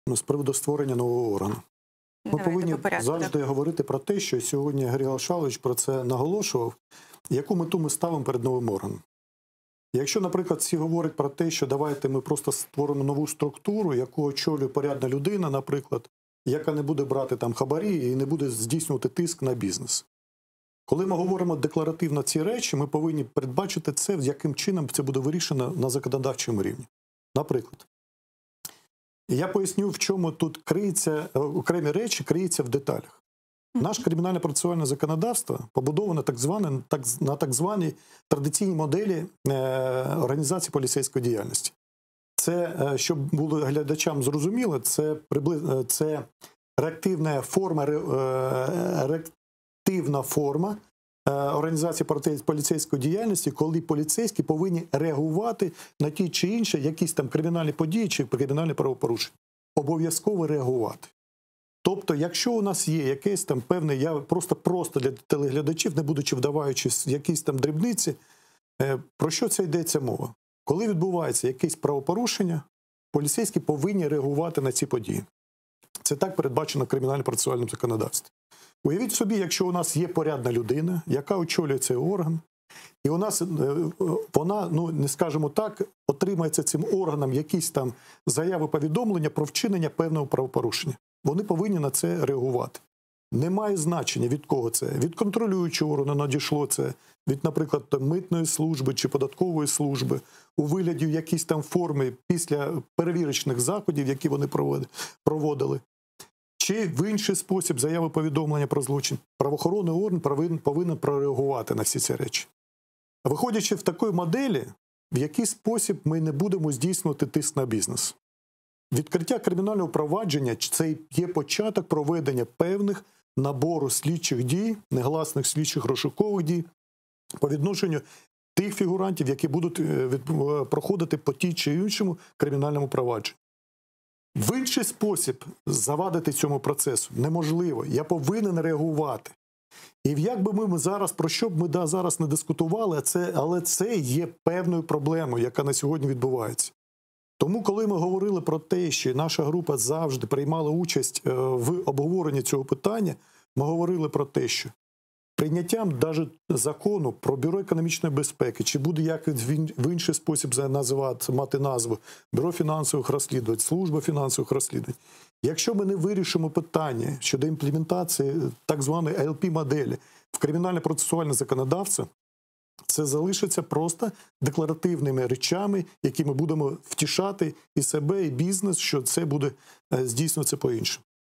Навіщо Україні Бюро економічної безпеки?
Обговорення законопроекту №30-87Д “Про Бюро економічної безпеки України”. Метою проекту визначено розділення сервісної та правоохоронної функції Податкової служби України, ліквідацію податкової міліції, оптимізацію структури та чисельності органів, які ведуть боротьбу зі злочинами у сфері фінансів, для того щоб усунути дублювання в їхніх повноваженнях.

Яка мета стоїть перед новим органом у сфері економічної безпеки?

Коментує В’ячеслав Некрасов - кандидат юридичних наук, доцент, радник Голови ДФС України, провідний науковий співробітник Національної академії внутрішніх справ